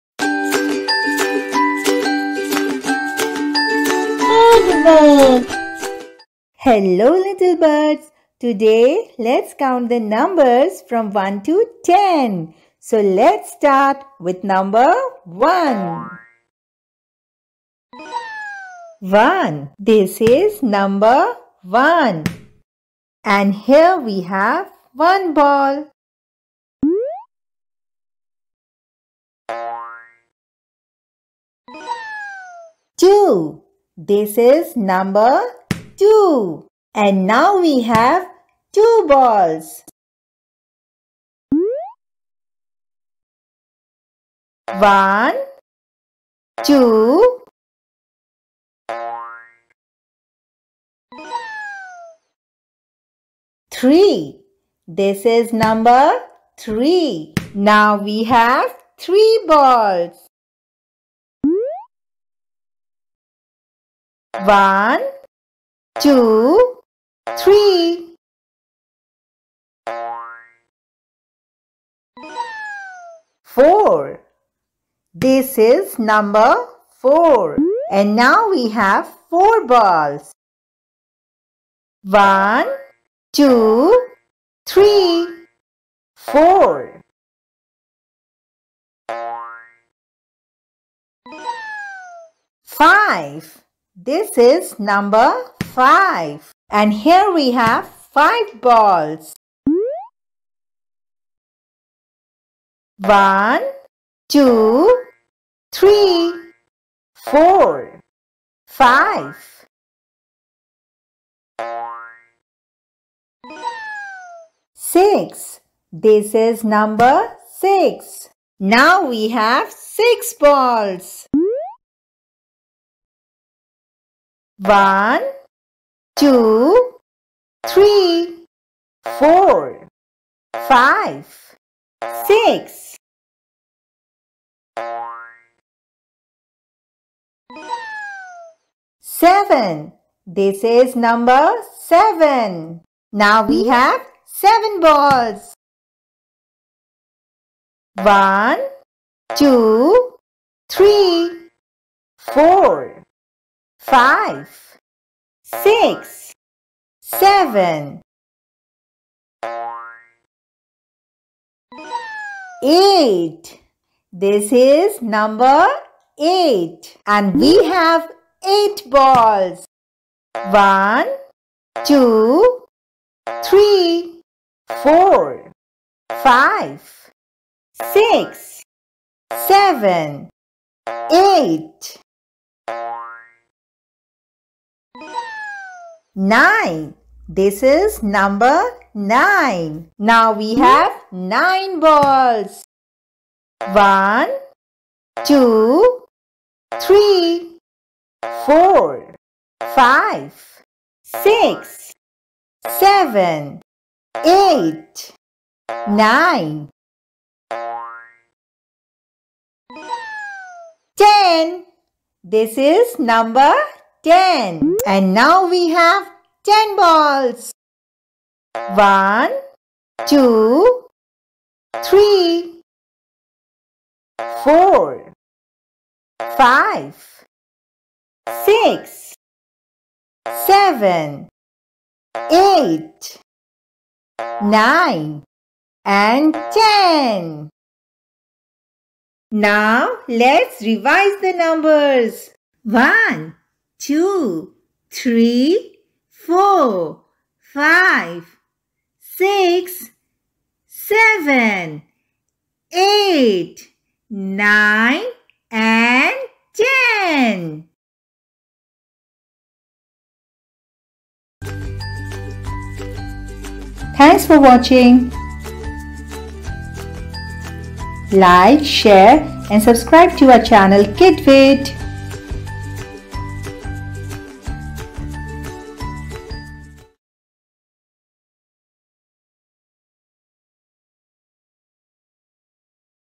Easy way to learn NUMBERS 1 To 10 For Kids | Count 123 with Colourful Balls | Pre School
Hello Little Buds!
Today Lets Count the Numbers from 1 To 10.
Toddlers, Babies and Kids love to see colors therefore we have used colourful  cartoon Numbers and Balls to make it an interesting and fun way to learn the numbers in the most easy way.
In this video, Numbers are being taught in a very exciting way which will help your babies to understand and learn the numbers quickly;
Not only your kids will be able to identify the numbers next time you ask them but will also help them to count the number of objects placed before them which is again very important.
The video is a delight to watch.
So lets start and lets learn and count the Numbers from 1 To 10!!